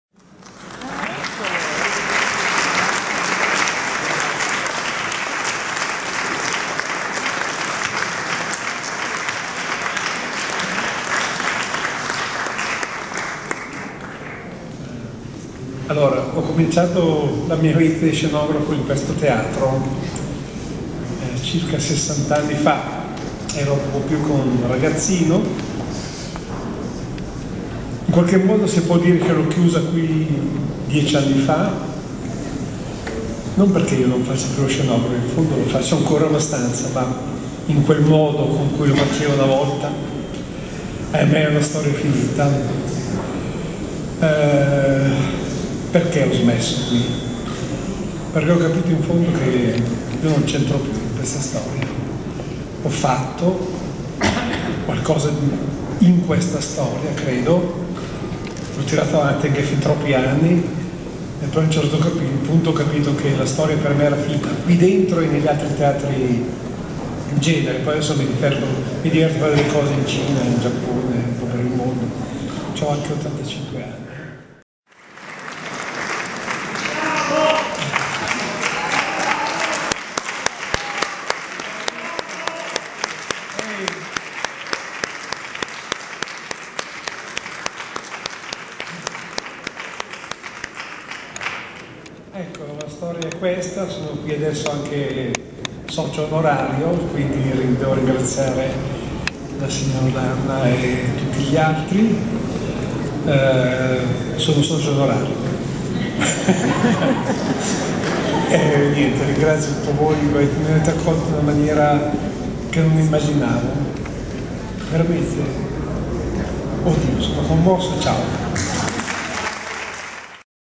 l'intervento commosso di Ezio Frigerio
Video registrato durante la presentazione dei nuovi libri degli Amici della Scala, tenutasi lo scorso 27 novembre 2014 al Teatro Alla Scala, Ridotto dei palchi A. Toscanini.